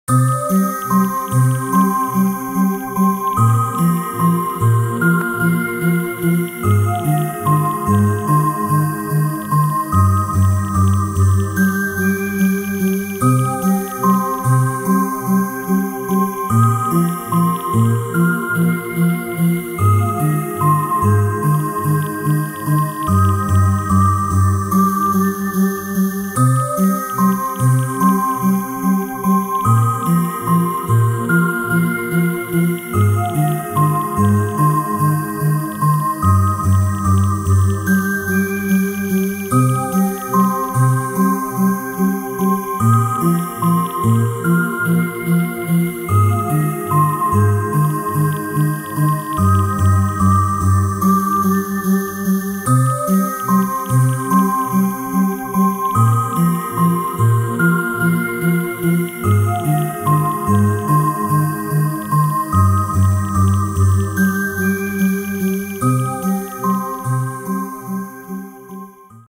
SPACE BALL
I have lost the dogs ball again